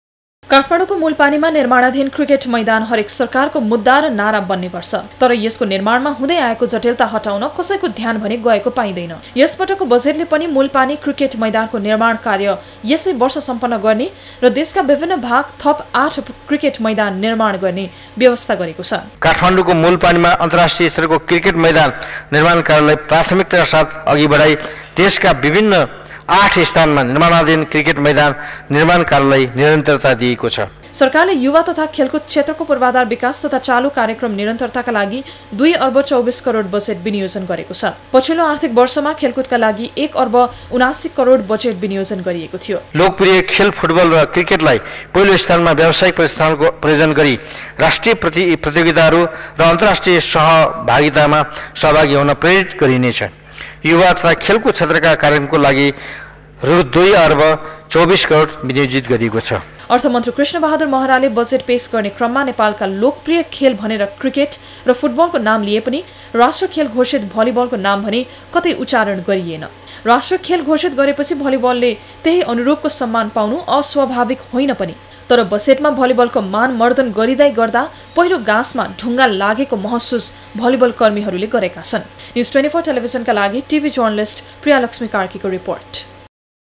आगामी आर्थिक बर्षको बजेटमा खेलकुद क्षेत्रलाई सम्मानजनक रकम विनियोजन-NEWS 24
सरकारले खेलकुद क्षेत्रका लागि पछिल्लो बर्षको तुलनामा बजेटमा सम्मानजनक वृद्धि गरेको छ भने क्रिकेट र फुटबलको विकासका लागि केही कार्यक्रमहरु अघि सारेको छ । तर भर्खरै मात्र राष्ट्रिय खेल घोषणा गरेको भलिबलको विषयमा भने बजेट चु सम्म पनि बोलेको छैन । काठमाडौंको मुलपानीमा निर्माणाधीन क्रिकेट मैदान हरेक सरकारको मुद्दा र नारा बन्ने गर्छ । तर यस्को निर्माणमा हुँदै आएको जटिलता हटाउन कसैको ध्यान गएको पाइदैन । यसपटकको बजेटले पनि मुलपानी क्रिकेट मैदानको निर्माण कार्य यसैबर्ष सम्पन्न गर्ने र देशका बिभिन्न भाग थप आठ क्रिकेट मैदान निर्माण गर्ने व्यवस्था गरेको छ ।